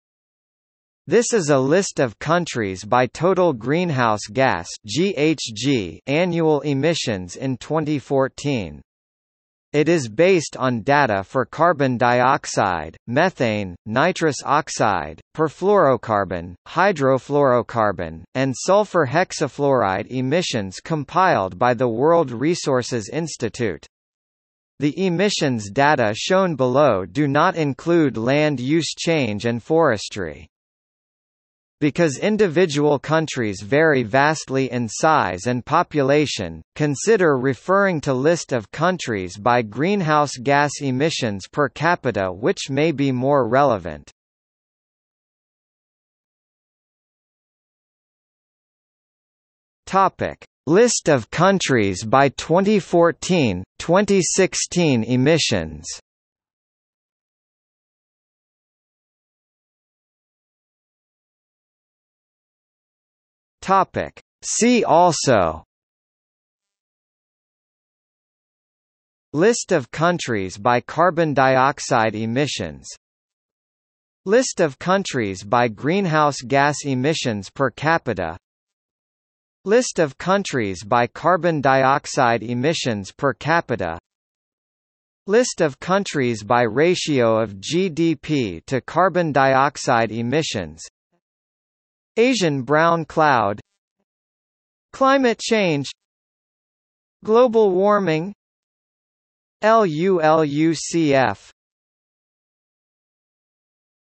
List of countries by greenhouse gas emissions | Wikipedia audio article
00:00:46 List of countries by 2014,2016 emissions
00:01:00 See also

- Socrates

SUMMARY
This is a list of countries by total greenhouse gas (GHG) annual emissions in 2014. It is based on data for carbon dioxide, methane, nitrous oxide, perfluorocarbon, hydrofluorocarbon, and sulfur hexafluoride emissions compiled by the World Resources Institute. The emissions data shown below do not include land-use change and forestry.
Because individual countries vary vastly in size and population, consider referring to List of countries by greenhouse gas emissions per capita which may be more relevant.